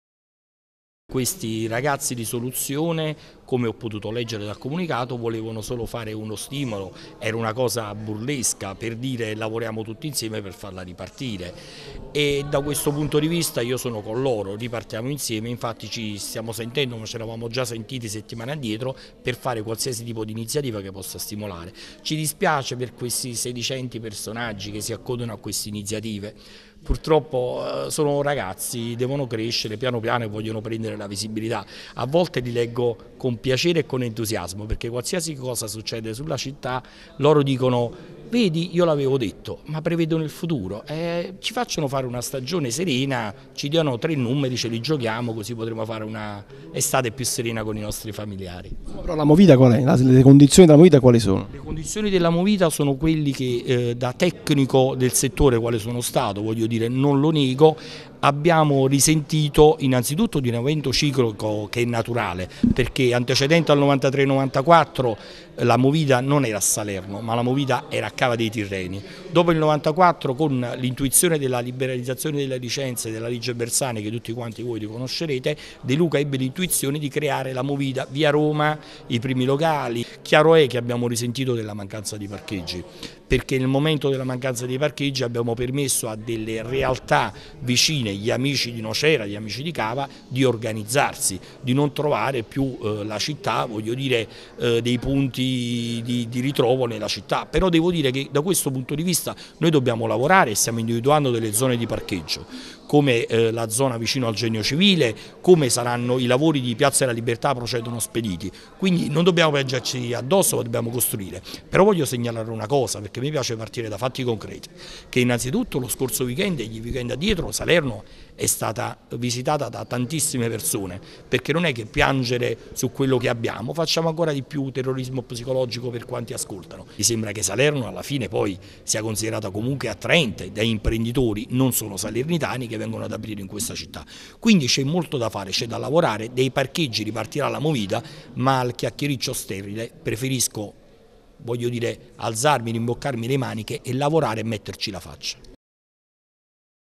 Loffredo e la veglia sulla Movida a Salerno: Iniziativa burlesca strumentalizzata
Sulla polemica sulla Movida c'è da registrare anche la presa di posizione dell'assessore all'annona del Comune di Salerno, Dario Loffredo che ha ammesso le difficoltà degli operatori per le quali però l'amministrazione si starebbe operando, bocciando il sostegno politico di Dante Santoro. (040517 Giancarlo Frasca)